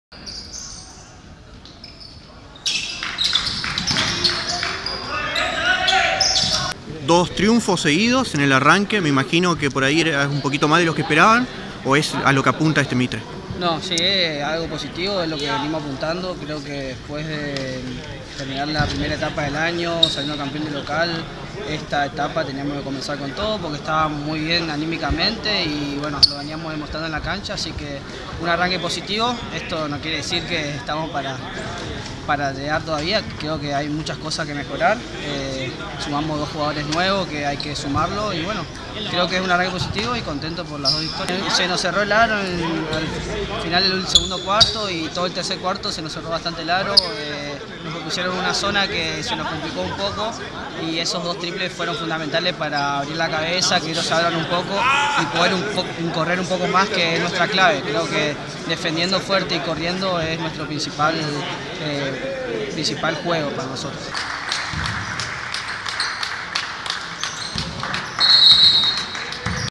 Francisco Stassi, Mitre - Mitre Basketball 65-54 Siglo XXI - Liga Provincial
El jugador de Mitre habló tras la segunda victoria del equipo en el Torneo Clausura de la Liga Provincial, y sostuvo "ésta etapa había que arrancar con todo por que estamos muy anímicamente..."

En el microestadio auriazul, Mitre Basketball venció a Siglo XXI por 65-54, en el cierre de la 2° fecha del Torneo Clausura 2019 en la Liga Provincial de Básquetbol.